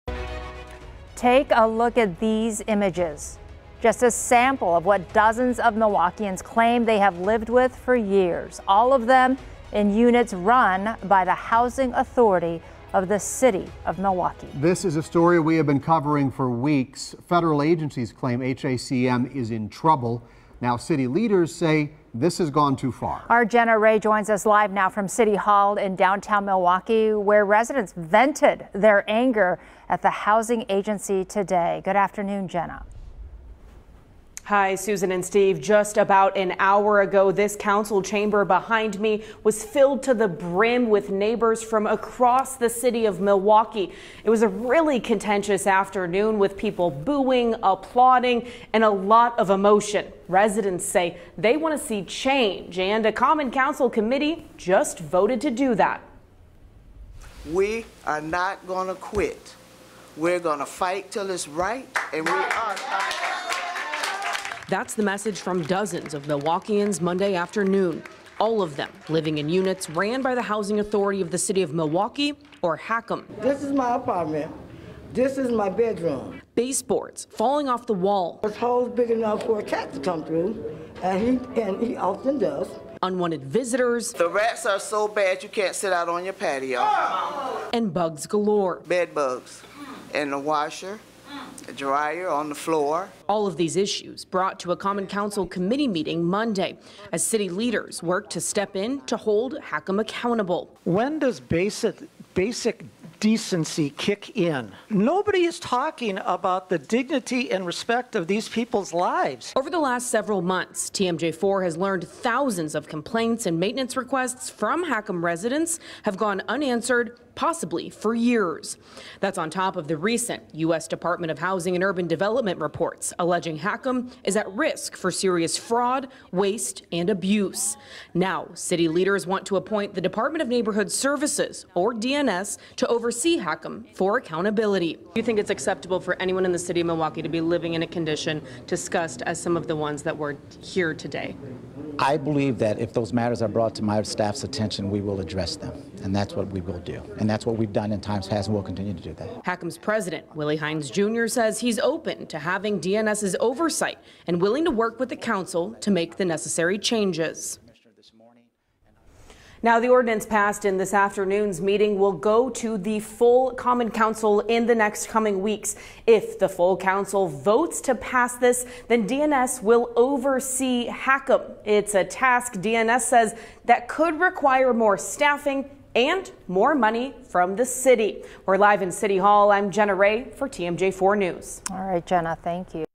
Complaints over poor living conditions in Milwaukee
On Monday, the Milwaukee City Common Council’s Steering & Rules Committee passed an ordinance that would allow the Department of Neighborhood Services (DNS) to oversee the Housing Authority of the City of Milwaukee (HACM).

The committee chamber was filled to the brim with neighbors across Milwaukee. It was a contentious afternoon filled with boos, applause and emotion. Residents say they want change, and the common council committee voted to do just that.

"We are not going to quit. We're going to fight until it's right," HACM resident, Vivian Jones, said.

That was the message from dozens of Milwaukeeans Monday afternoon. All of them live in units run by HACM.